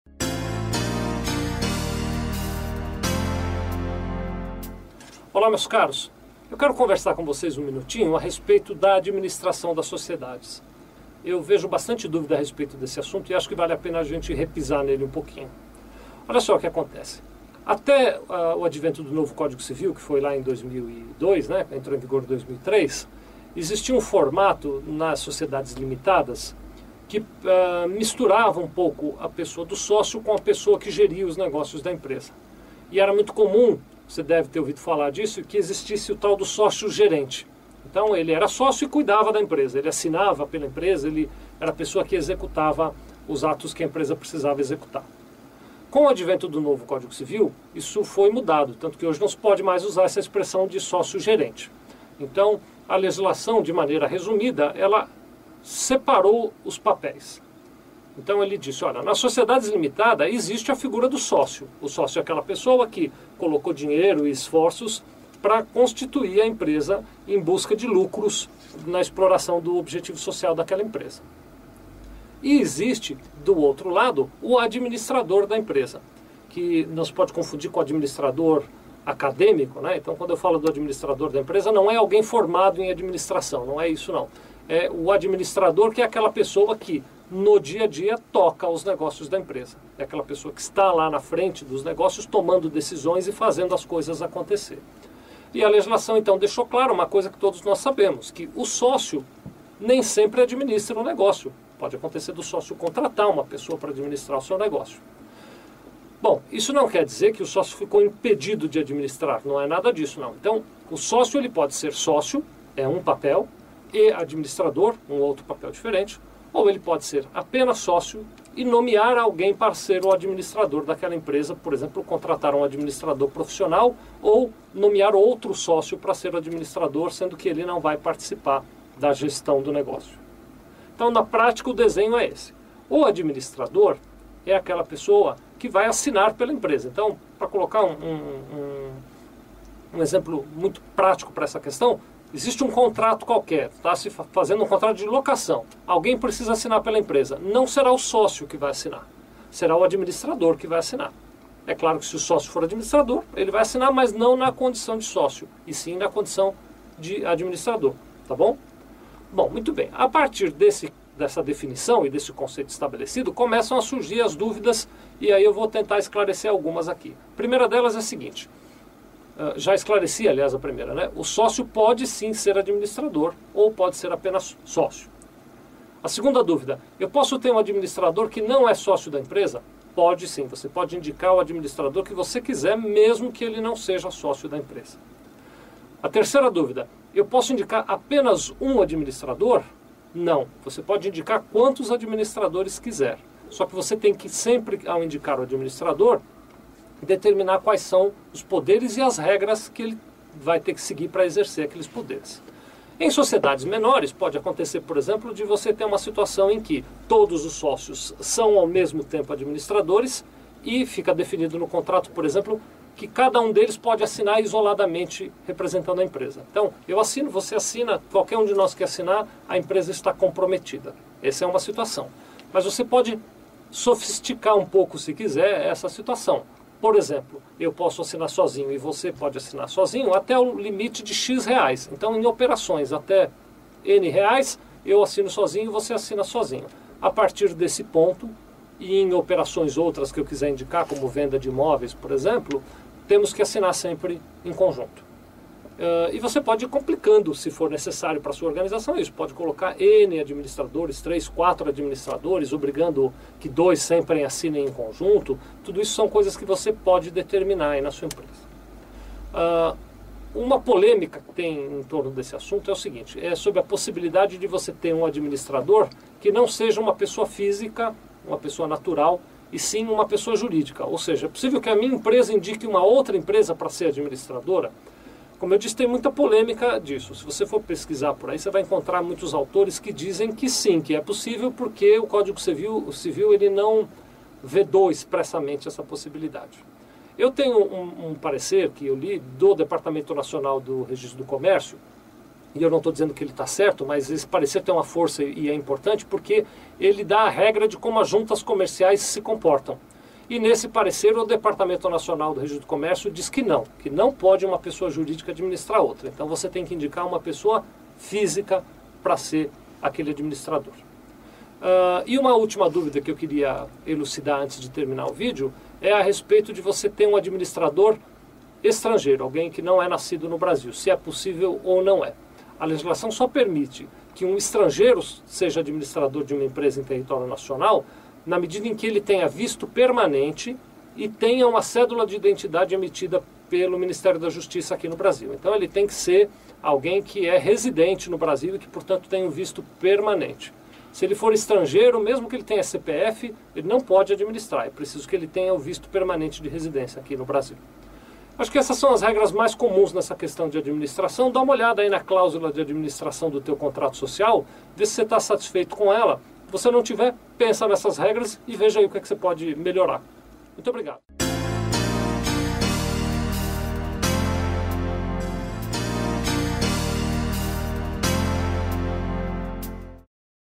Administração da Sociedade -- Sevilha Contabilidade
Administração da Sociedade -- Sevilha Contabilidade -- Vicente Sevilha Junior
Conheça em detalhes as regras para se nomear um administrador de uma sociedade.